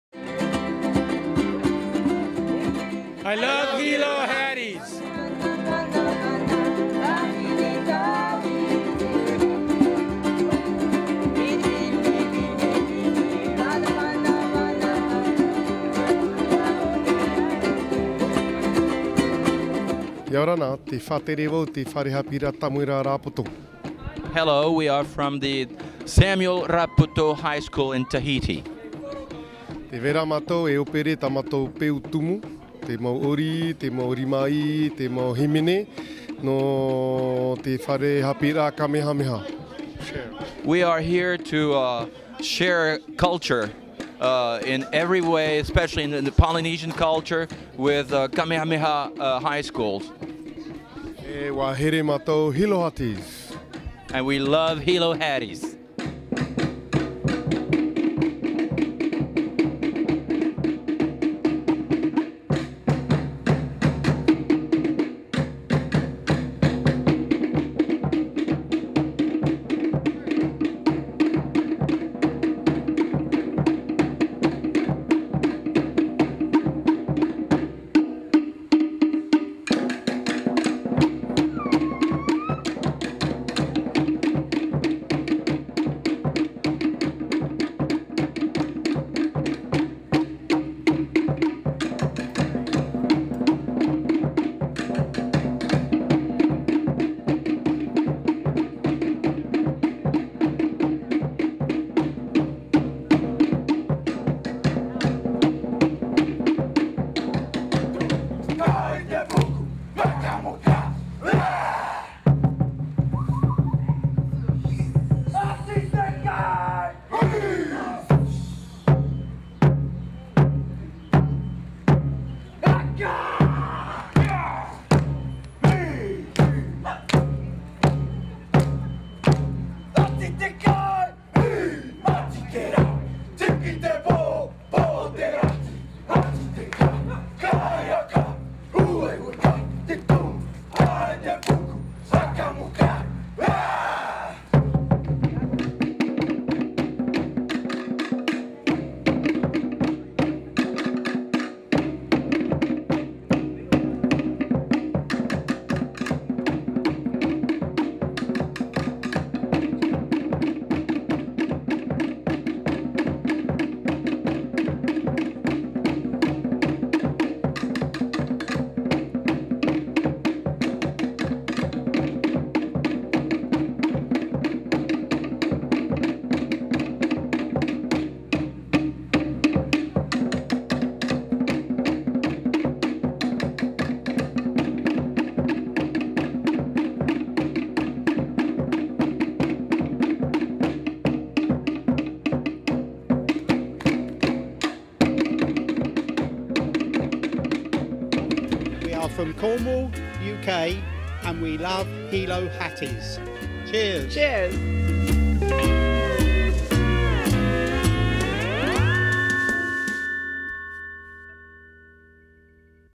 Jean Pierre Sartore Devasse Raapoto School Hilo Hattie Outlet Lahaina
Interview with Jean Pierre Sartore Devasse, director of the Lycee Samuel Raapoto School of Tahiti. Includes a performance from the Lycee Samuel Raapoto School of Tahiti at the Hilo Hattie Outlet at the Outlets of Maui Lahaina. Filmed and Edited by Frank W. Pulaski III, DreamTyme Films